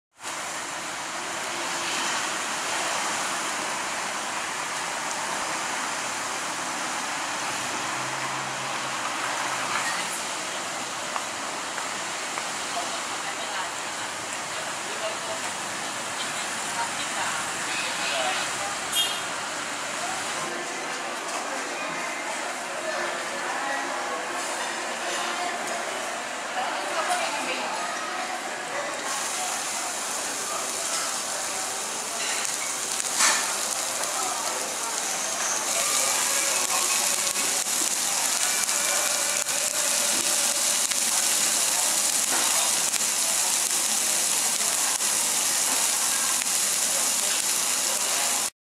Sizzler-Royal Garden Plaza Pattaya(รอยัล การ์เด้น พลาซ่า พัทยา)
If you order main dish, a salad bar follows. The salad bar is rich in variety and you can eat live vegetables abundantly in Thailand where there is not a chance to eat easily. There are also soup, fruit and dessert. I think steak is good when I am tired of Thai food in the main restaurant.